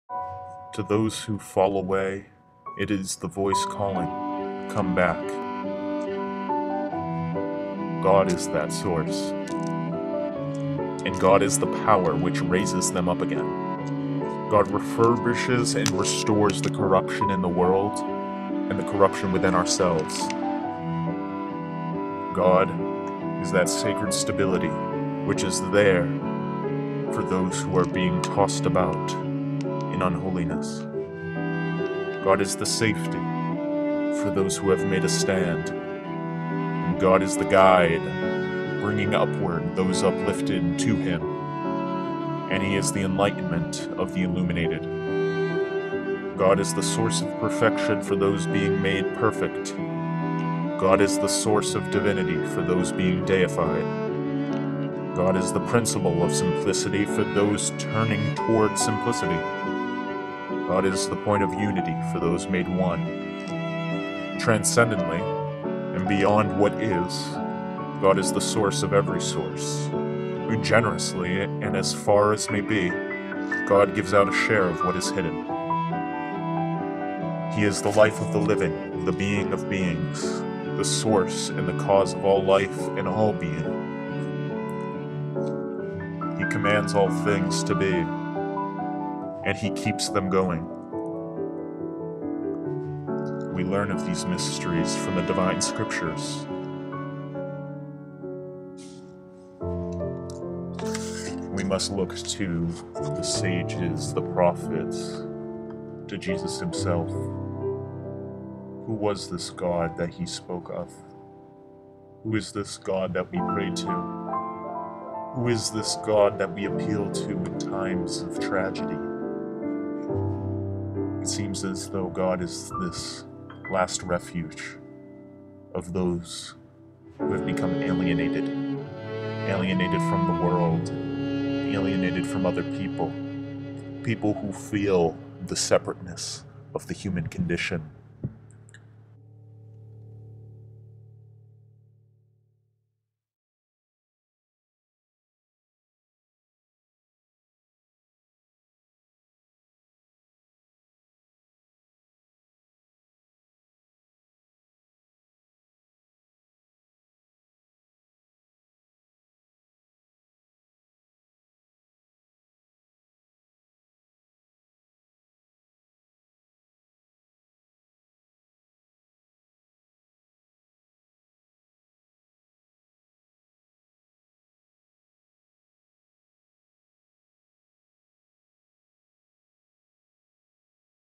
What is That which we call God?
It is silly to ask whether God exists. If we wish to discover God, we must attend to what has been said. Thankfully we have lots of books and saints to look up to.
This is my reading of some of Dionysius the Areopagite— Divine Names. Dionysius is universally regarded as the best, so it can't hurt to read him in our search for That which matters most.
I hope 'God' will continue to hold a large place within our channel. — Kevin